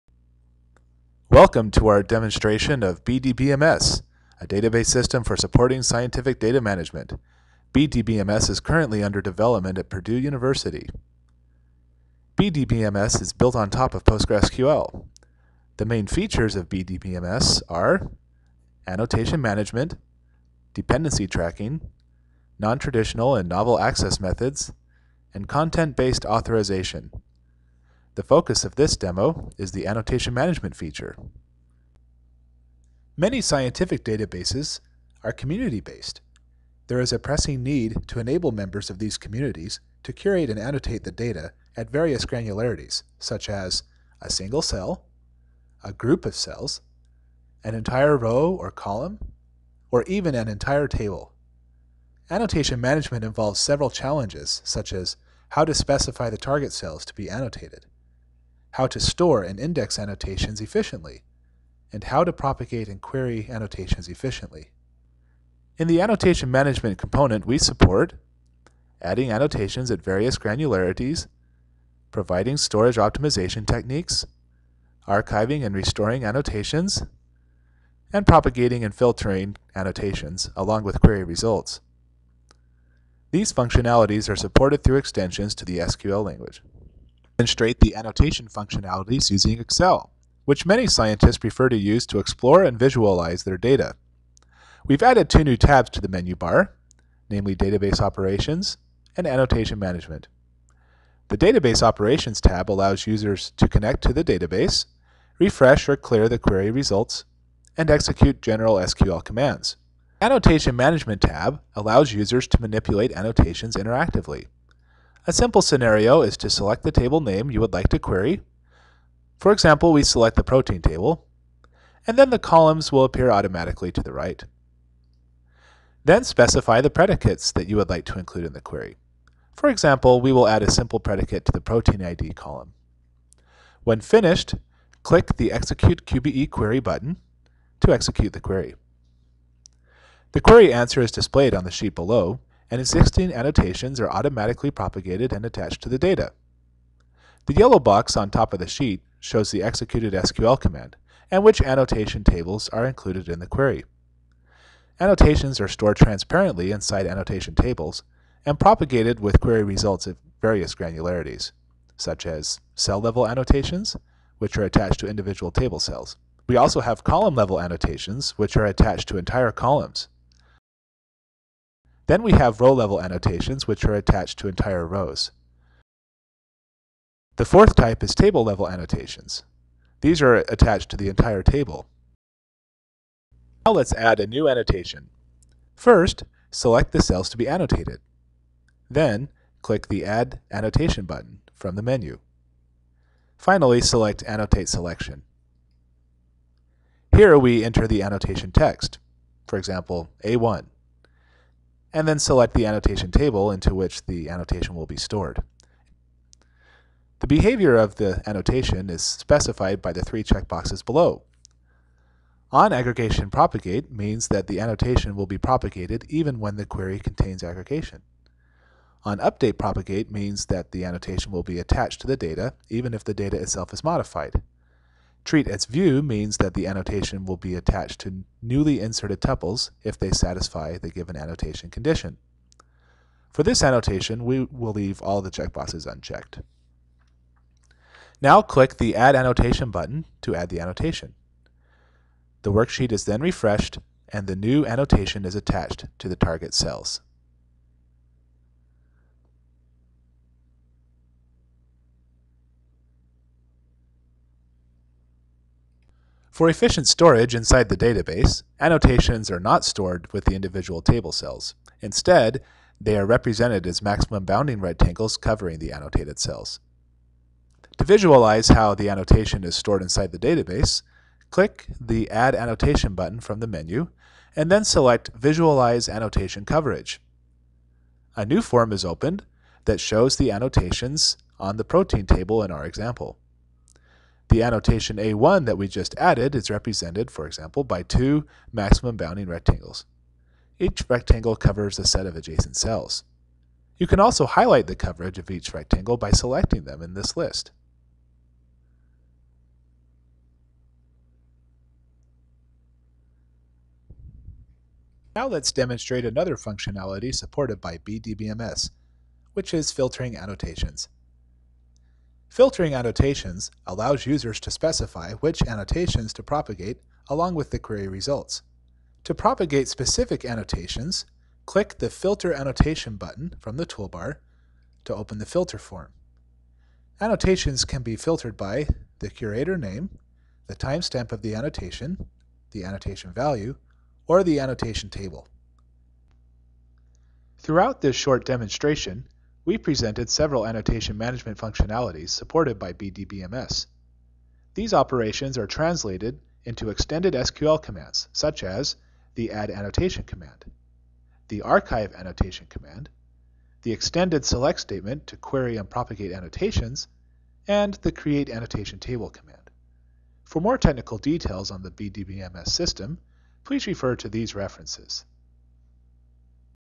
bdbms: A Database System for Scientific Data Management
bdbms is an extended database system for supporting scientific data management. bdbms is under development at Purdue University, IN, USA.

Group members are:
Mohamed Eltabakh,   Mourad Ouzzani ,  Walid Aref ,  Ahmed Elmagarmid, Yasin Silva,  Umer Arshad,  David Salt,  Ivan Baxter